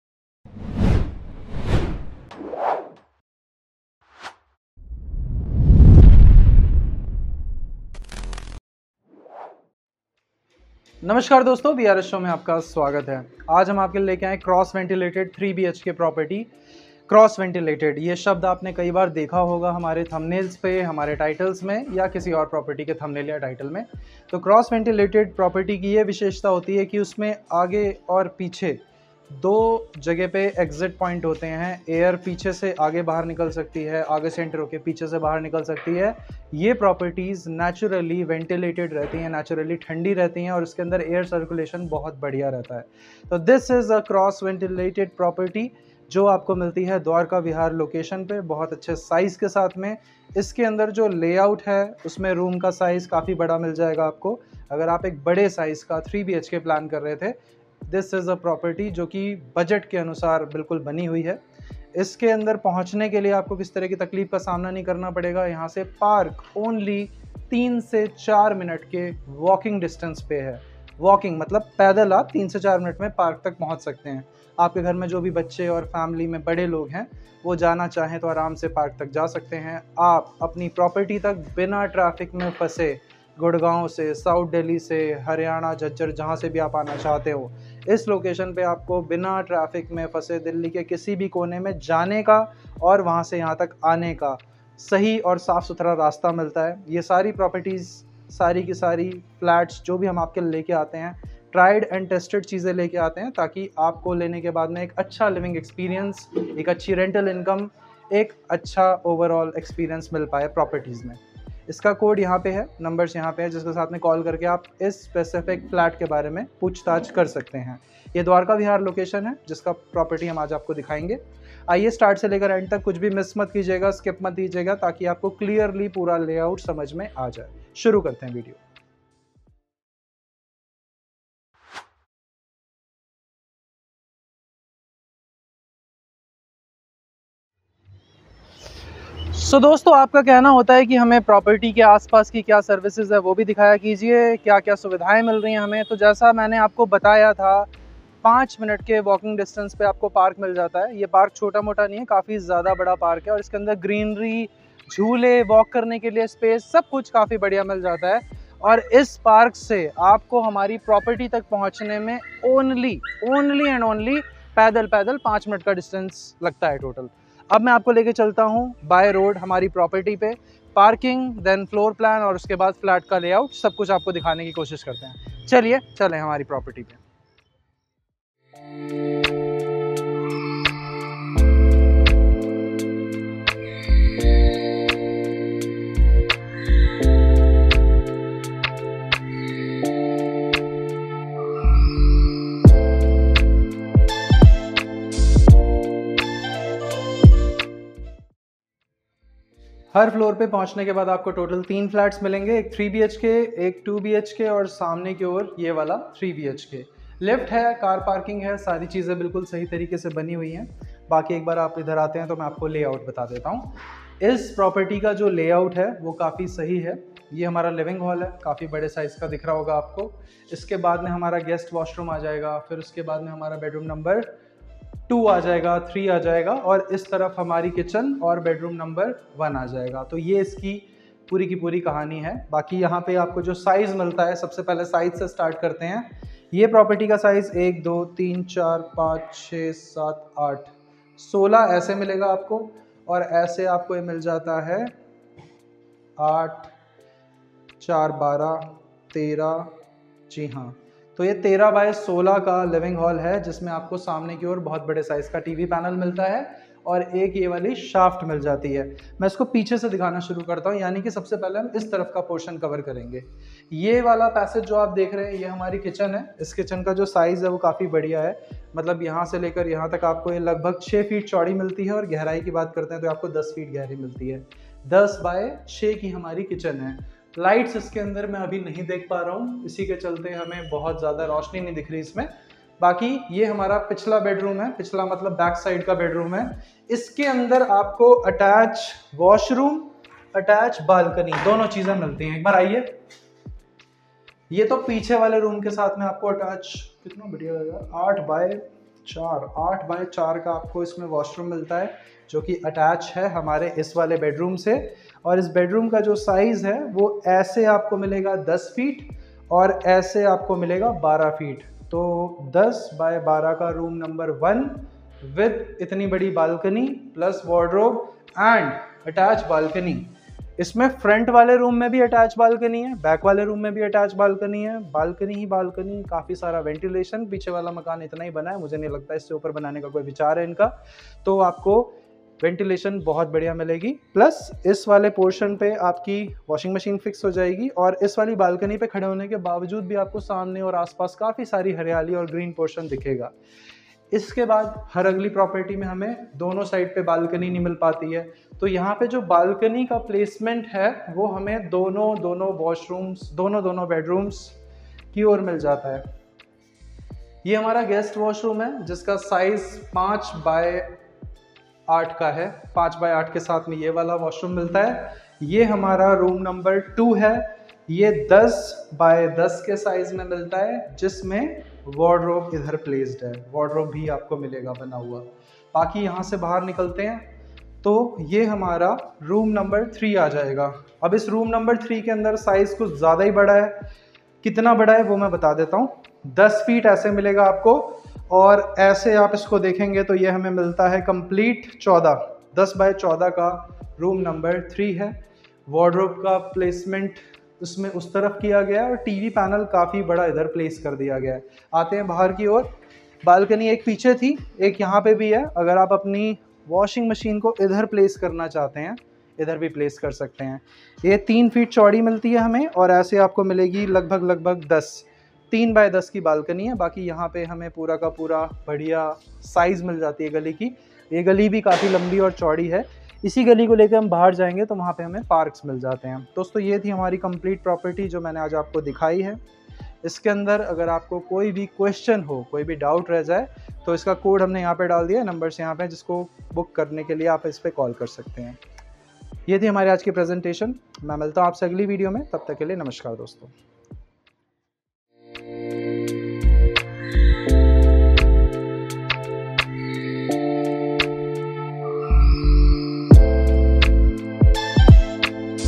100 गज का 3 BHK Flat for sale only 0.2 Km From PARK 🌳in 55 Lacs 📍 DWARKA VIHAR | BRS SHOW S490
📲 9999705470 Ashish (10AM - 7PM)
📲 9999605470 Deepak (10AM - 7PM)

* Property Code - BRS SHOW S490
* Price - 55 - 60 Lacs.
* Size  -  900 Sq. Ft.
* Loanable - Up to 80%
* Floor - 1 [ P 1 2 3 4 ]
* Car Parking - Four Wheeler Parking.
* Lift - Yes
* Location - Dwarka Vihar
* DJB (Delhi Jal Board Water Connection).
* School 200 Meter
* Hospital 500 Meter
* Local Market nearby.

Head office - BRS REAL ESTATES
S8/C Vishwas Park Extension.
Opposite - Punjab National bank Rajapuri Sector 3 Dwarka